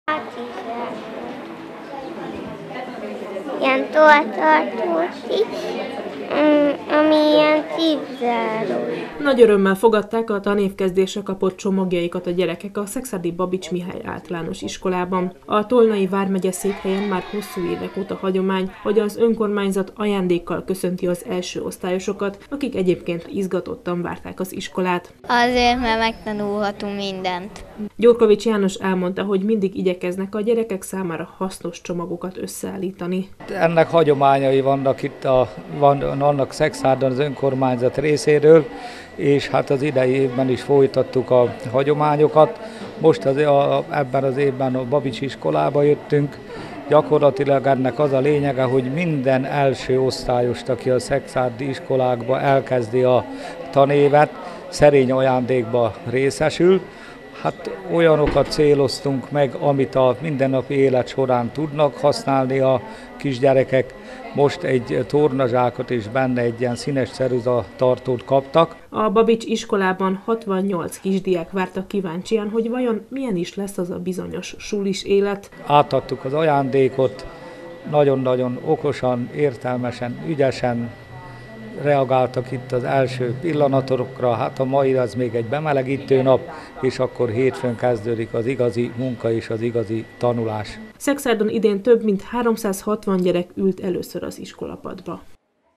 Ajándékot kaptak az elsősök - több mint 360 kisdiák kezdte meg tanulmányait Szekszárdon
A hagyományokhoz híven az idei évben is megajándékozták azt több, mint 360 első osztályos diákot, akik megkezdték tanulmányaikat Szekszárd általános iskoláiban. A csomagokat Gyurkovics János alpolgármester osztotta ki.